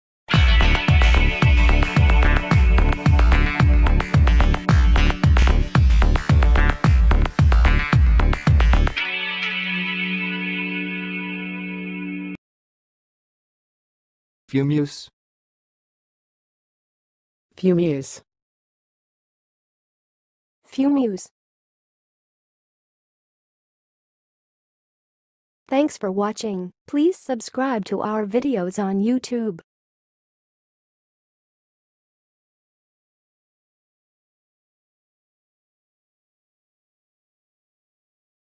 FUMEUSE - How to say Fumeuse?
FUMEUSE - Say Fumeuse like an expert: Pronunciation guide in 2023

Une fumeuse est une personne qui fume régulièrement du tabac ou d'autres substances.

Pronunciation:
Pour prononcer 'fumeuse', vous pouvez dire [fy.møz] ou [fy.møzə]

Test yourself by speaking the following examples:
1. Elle est une fumeuse depuis plus de dix ans
2. Les fumeuses ont un risque plus élevé de développer des maladies pulmonaires
3. Il existe de nombreuses méthodes pour aider les fumeuses à arrêter de fumer
4. La fumeuse est consciente des dangers du tabagisme
5. Certaines fumeuses préfèrent utiliser une cigarette électronique pour réduire leur consommation de tabac
6. La fumeuse a décidé d'arrêter de fumer et a utilisé des patchs nicotiniques pour l'aider